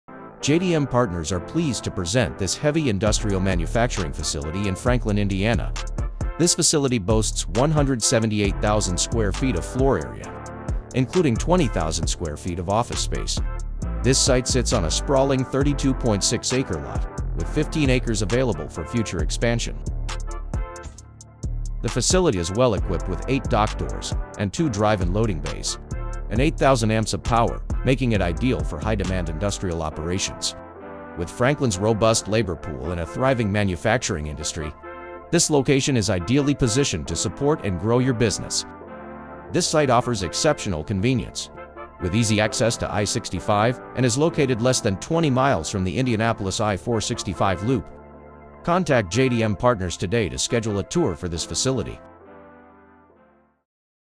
Prime Manufacturing Building for Sale or Lease | 1450 Commerce Parkway, Franklin, IN
Explore this premier manufacturing and warehouse facility located at 1450 Commerce Parkway, Franklin, IN. Spanning 178,060 square feet on a 32.6-acre lot, this property is available FOR SALE at $12,000,000 or FOR LEASE at $3.95 PSF NNN.

Built in 1994 and situated in Johnson County’s industrial hub, this versatile space is perfect for businesses seeking substantial warehouse and office areas with room for expansion. Key features include:

• 20,000 SF of Office Space: Customizable to meet diverse operational needs.

• Heavy Power: Equipped with 8,000 AMPs, ideal for high-demand industrial use.

• Multiple Loading Options: Includes both dock and drive-in loading.

• Expansion Potential: 15 acres available for additional parking or further development.

Don't miss this opportunity to enhance your business operations in a prime location.